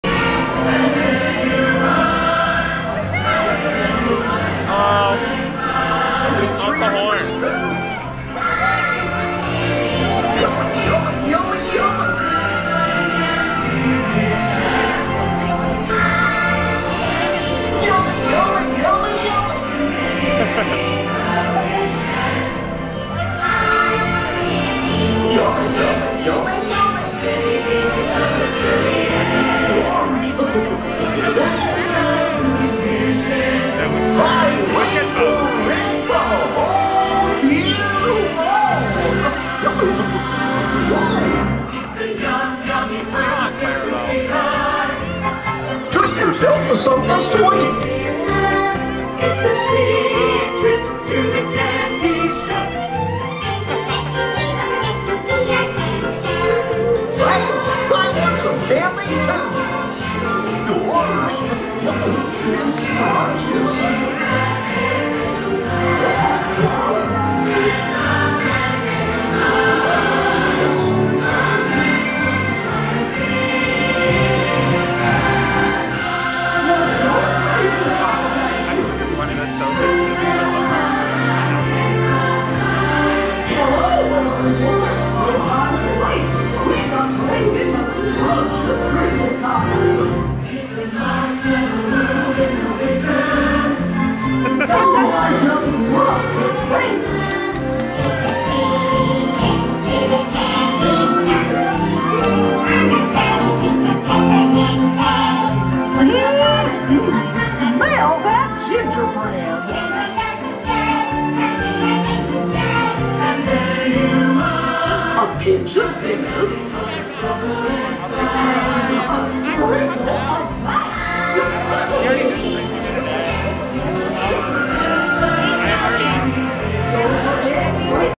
Goofy’s Scrumptious Cavalcade Night Version
ウォルトディズニーワールド、マジックキングダムの11/6から始まったソーシャルディスタンスをしながらのクリスマスパレード。(夜バージョン)
登場キャラクター：マックス、グーフィー